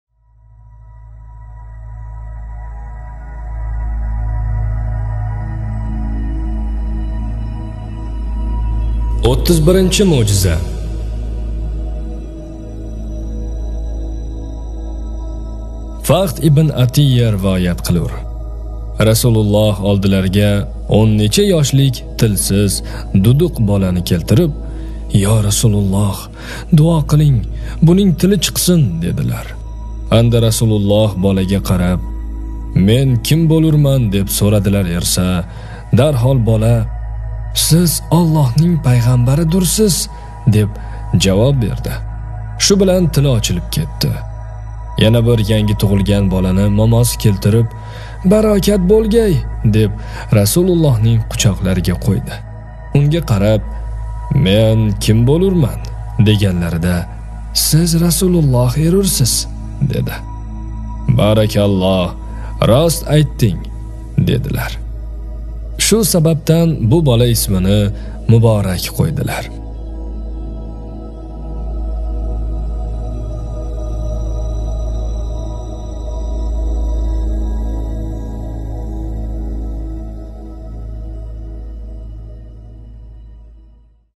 147. O'ttiz birinchi mo'jiza
"Tarixi Muhammadiy"
Muallif: Alixonto'ra Sog'uniy
O'qib beruvchi: Najot Nur